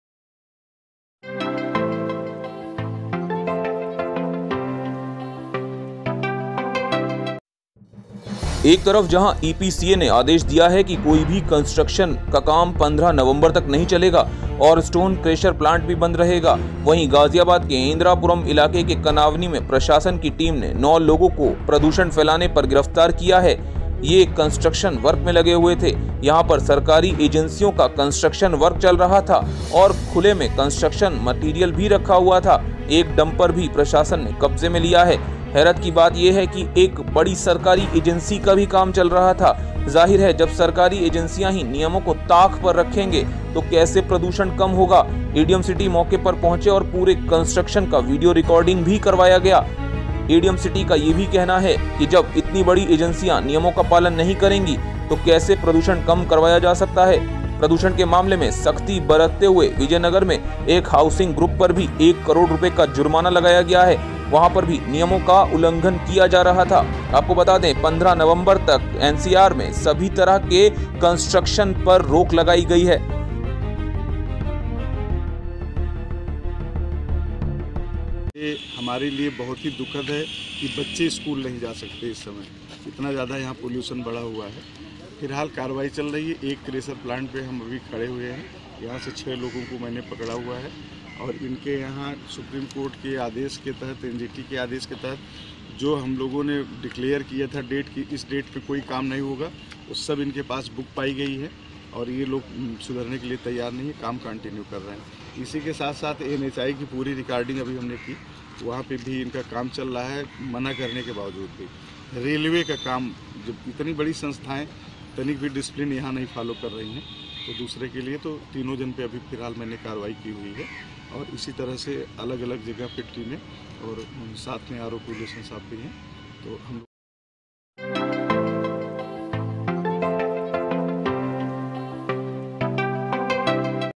Ghaziabad: प्रदूषण के मामले में सरकारी एजेंसी दिखा रही प्रशासन को ठेंगा
एक तरफ जहां  EPCA ने आदेश दिया है कि कोई भी कंस्ट्रक्शन का काम  15 नवंबर तक नहीं चलेगा। और स्टोन क्रेशर प्लांट भी बंद रहेगा। वही गाजियाबाद के इंदिरापुरम इलाके के कानावनी में प्रशासन की टीम ने  9 लोगों को गिरफ्तार किया है। यहां पर सरकारी एजेंसियों का काम चल रहा था।और खुले में कंस्ट्रक्शन मैटेरियल रखा हुआ था। एक डम्पर भी प्रशासन ने कब्जे में लिया है। हैरत की बात यह है कि बहुत बड़ी सरकारी एजेंसी का भी निर्माण का काम चल रहा था।जाहिर है जब सरकारी एजेंसियां ही नियमों को ताख पर रखेंगे,तो कैसे प्रदूषण कम होगा। एडीएम सिटी मौके पर पहुंचे और पूरे कंस्ट्रक्शन की वीडियो रिकॉर्डिंग भी करवाई गई।